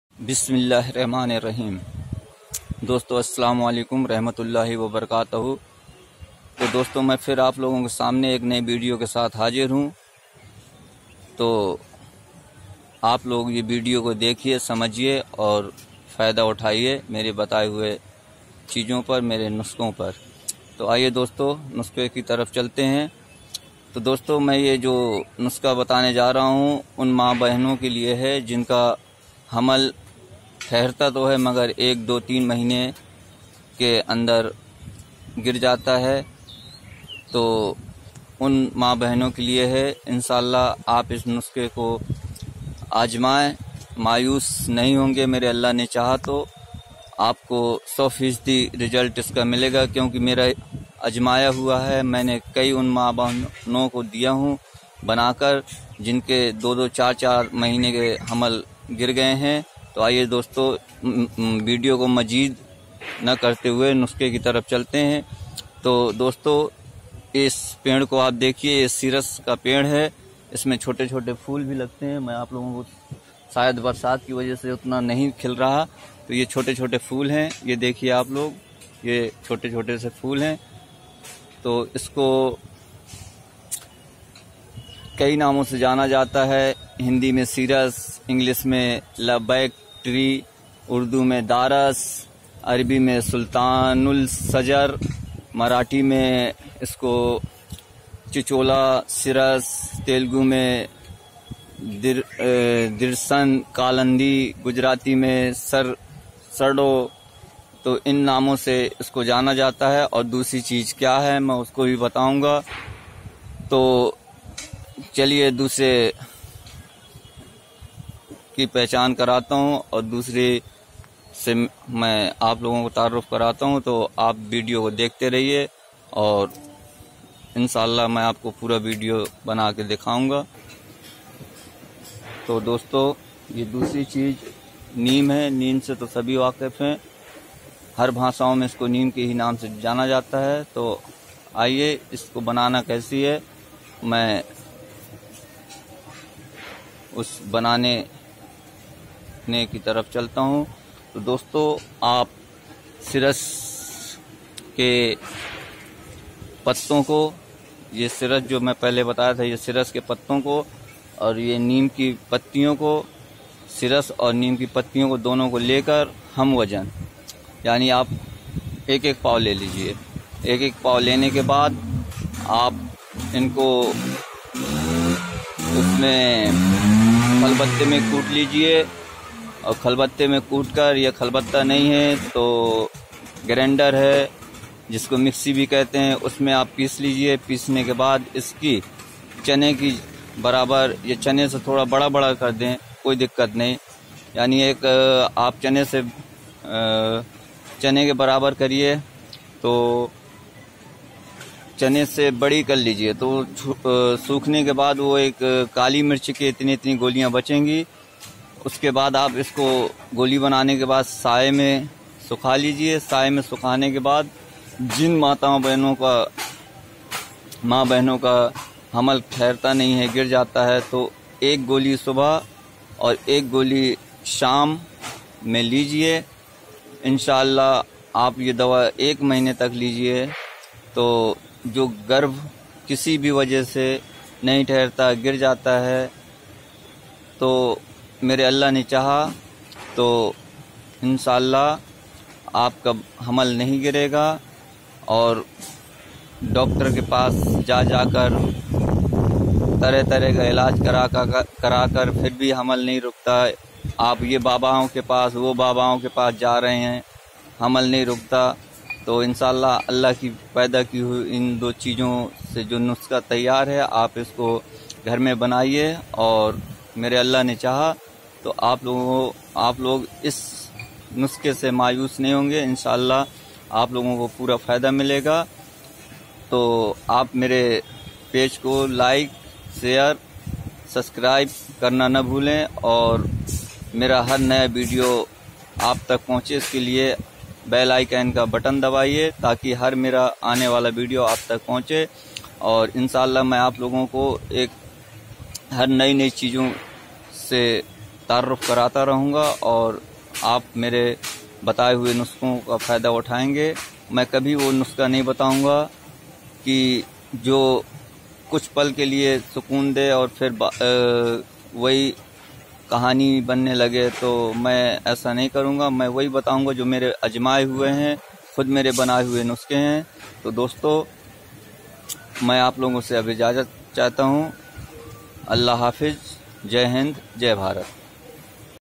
Jin Aurto ka Bar Bar Garbh Gir Jata Ho Unke Liye Ye Nuskha Tohfa hai
Health Zindagi Abdul Khan

Raipur Chhattisgarh India
calling 12:00  p.m. to 10:00 p.m.